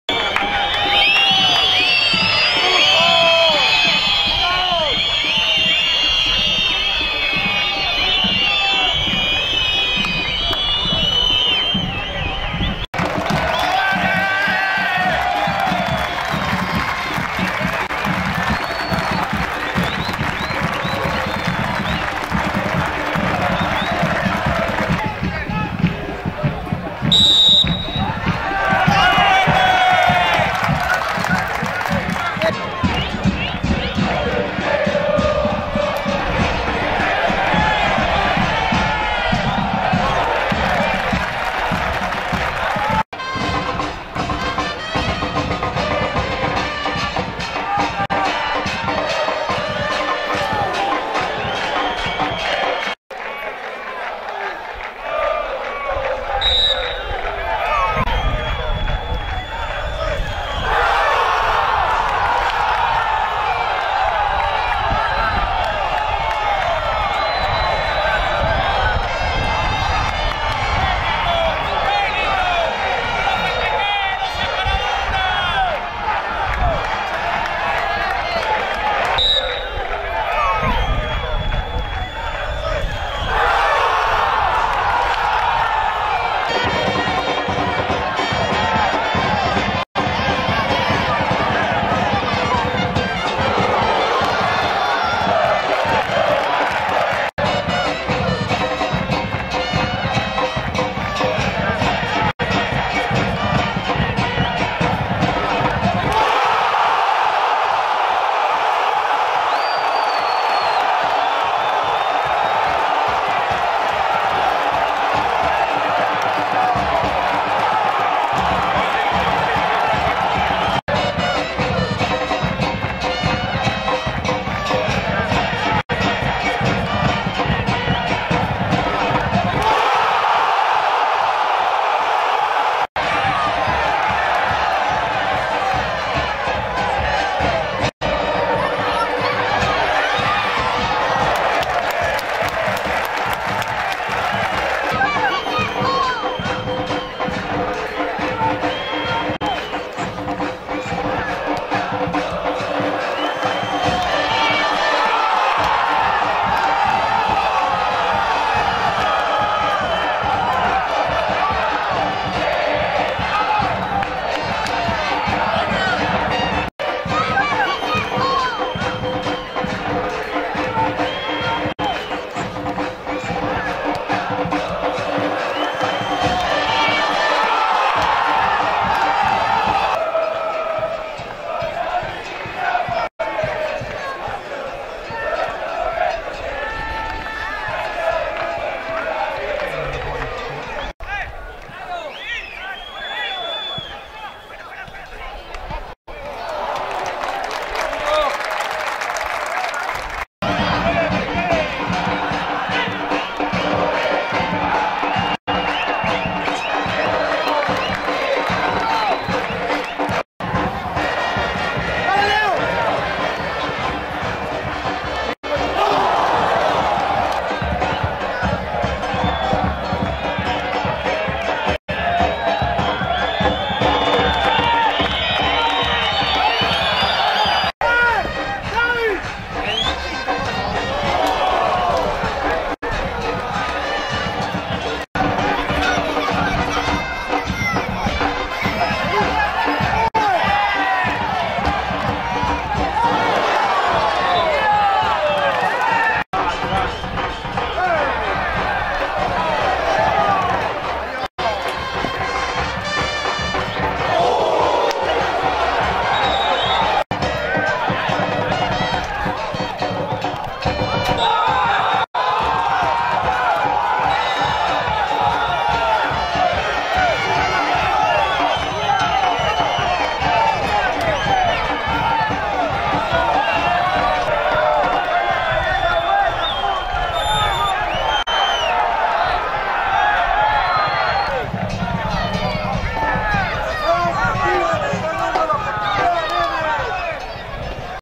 San Martín (SJ) 1-2 Racing (C) | Primera Nacional | Fecha 38 (Zona A)
Goles de Gonzalo Rostagno (RAC) a los 13', Nazareno Funez (SSJ) a los 17' y Bruno Nasta (RAC) a los 42'.